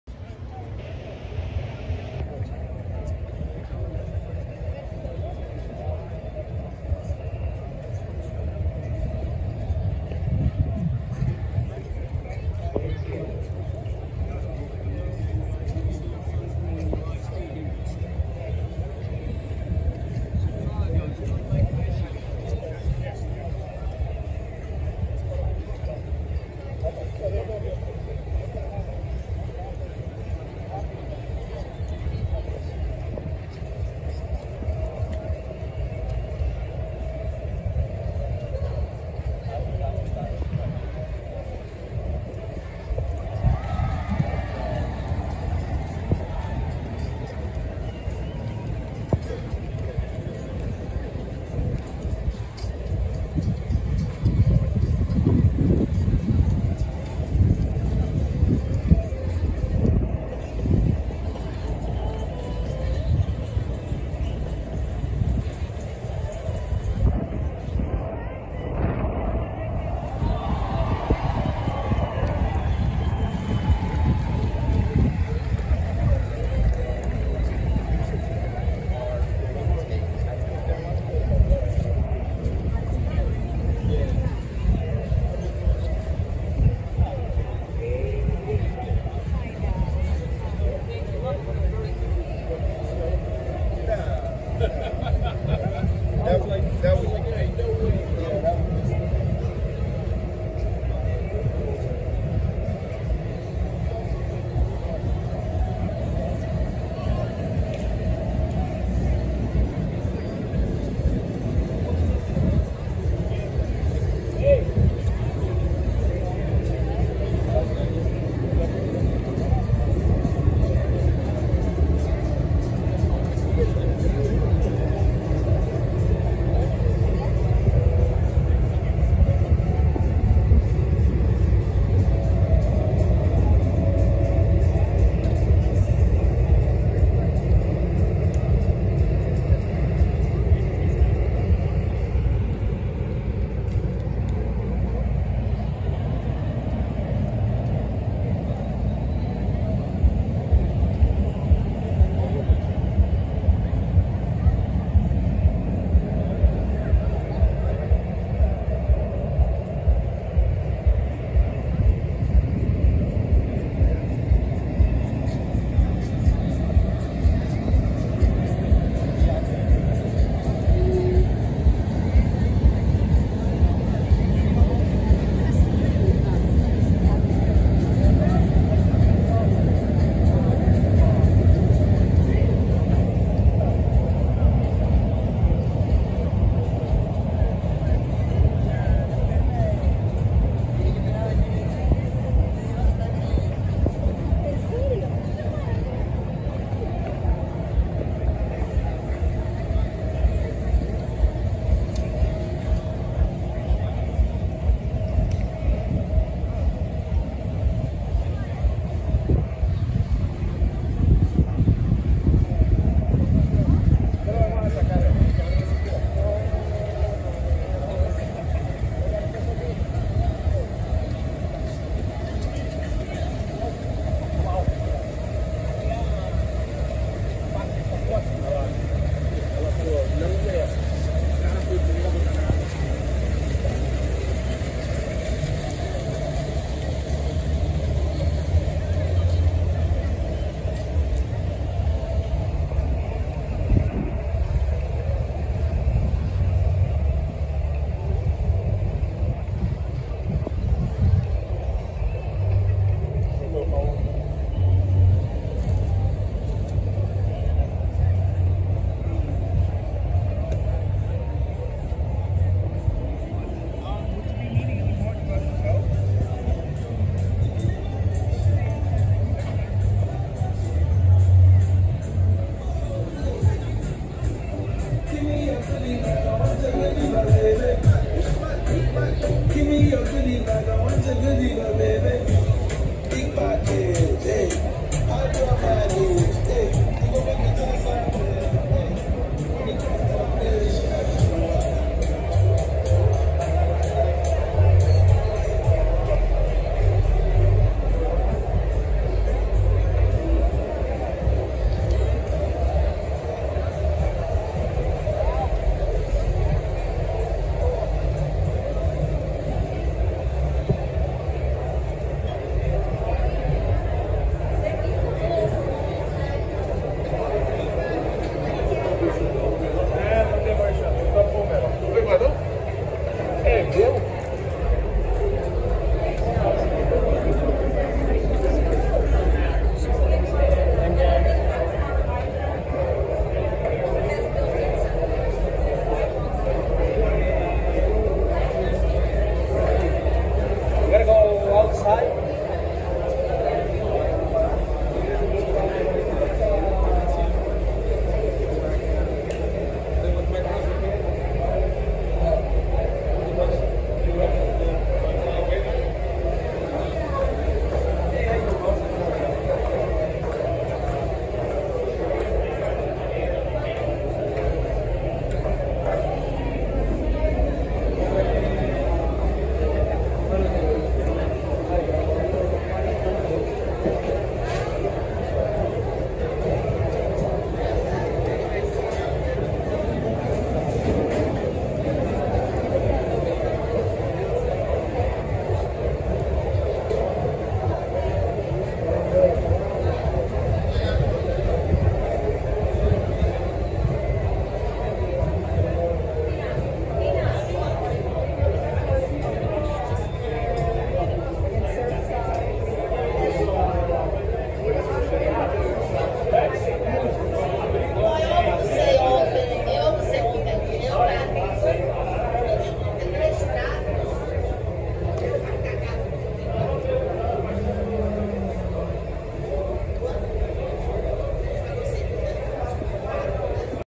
Miami Open 2022 Grounds Walking Tour
A walk around the grounds at the 2022 Miami Open on Wednesday, March 23, the first day of main draw play